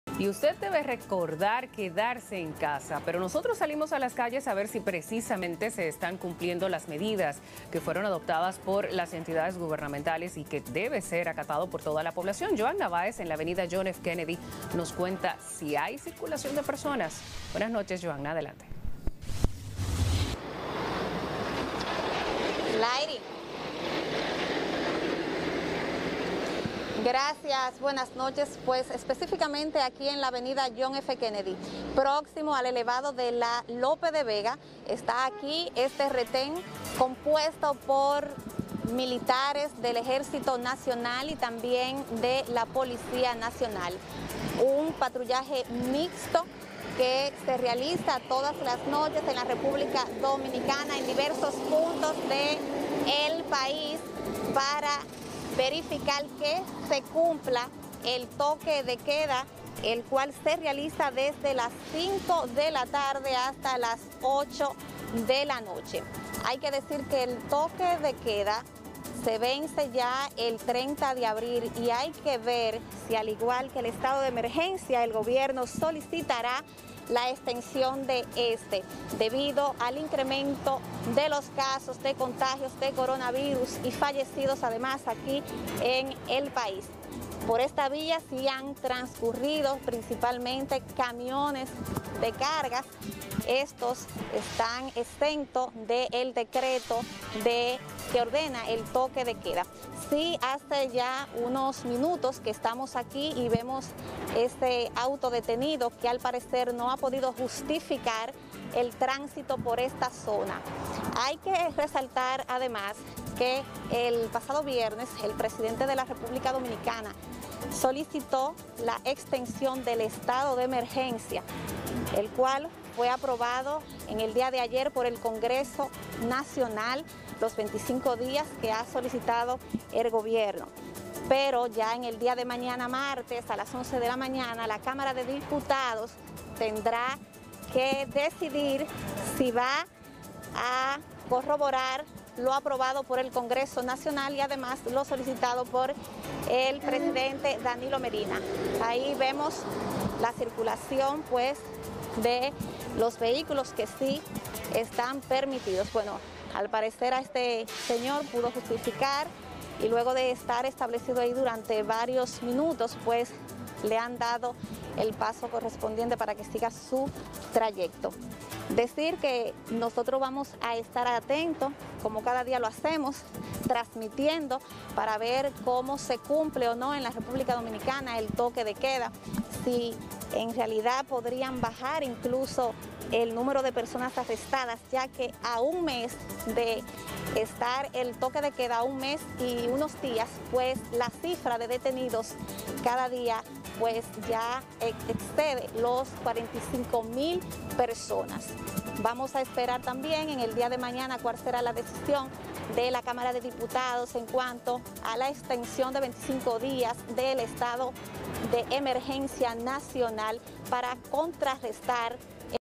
¿Dominicanos están cumpliendo el toque de queda?
Usted debe recordar quedarse en casa.

Salimos a las calles para revisar si se están cumpliendo las medidas adoptadas por las autoridades y que deben ser acatados por toda la población.